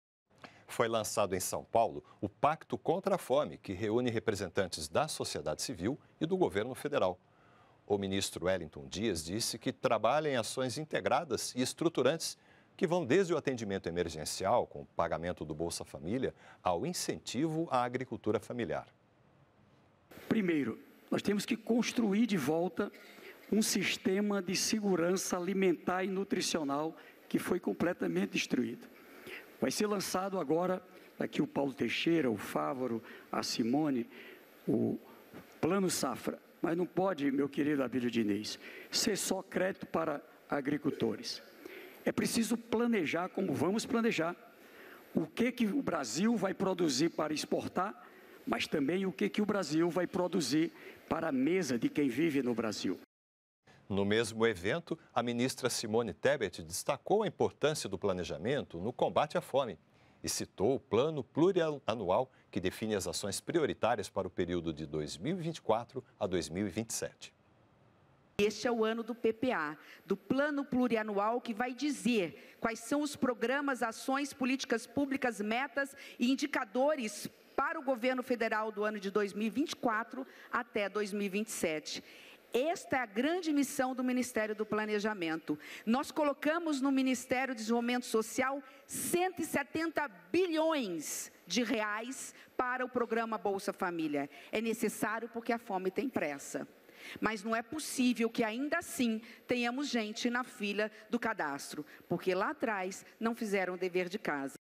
GOVERNO DEBATE AÇÕES DE COMBATE À FOME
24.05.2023 - Foi lançado, em São Paulo, o Pacto contra a Fome, que reúne representantes da sociedade civil e do governo federal. O ministro Wellington Dias disse que trabalha em ações integradas e estruturantes que vão desde o atendimento emergencial, com o pagamento do Bolsa Família, ao incentivo à agricultura familiar. No mesmo evento, a ministra Simone Tebet destacou a importância do planejamento no combate à fome e citou o Plano Plurianual, que define as ações prioritárias para o período de 2024 a 2027.